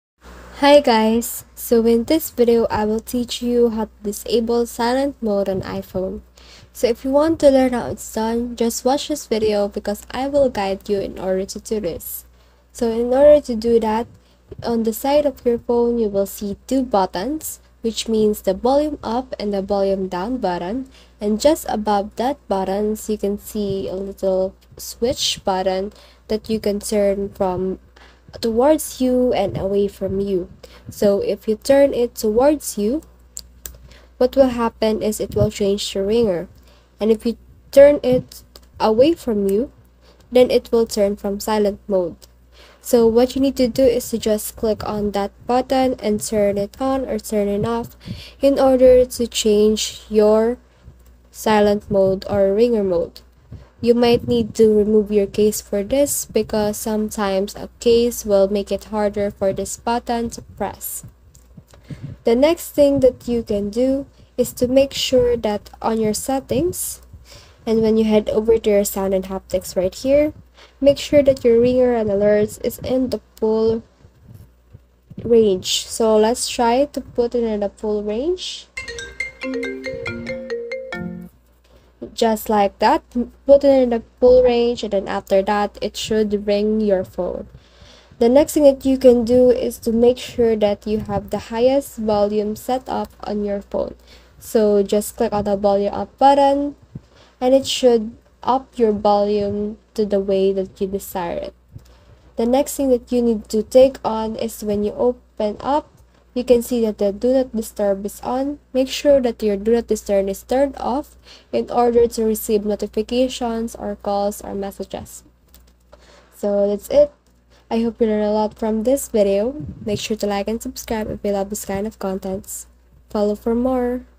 How To Disable Silent Mode Iphone [TUTORIAL]
How To Disable Silent Mode Iphone

In this video I'll show you How To Disable Silent Mode Iphone. The method is very simple and clearly described in the video. Follow all of the steps to disable the iPhone silent mode.